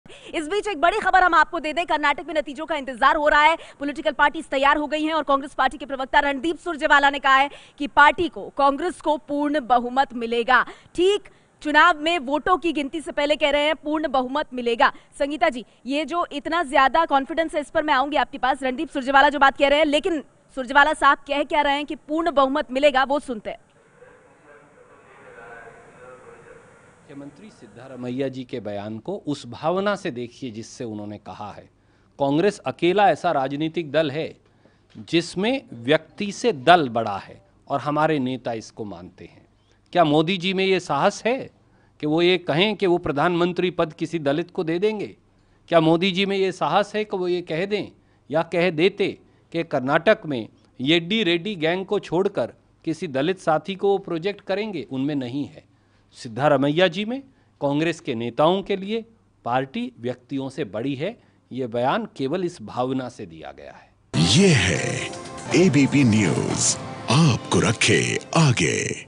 कर्नाटक चुनाव: कांग्रेस प्रवक्ता रणदीप सुरजेवाला का दावा- पार्टी को पूर्ण बहुमत मिलेगा | ABP Hindi
कर्नाटक चुनाव: नतीजों से पहले रणदीप सुरजेवाला का दावा- कांग्रेस को पूर्ण बहुमत मिलेगा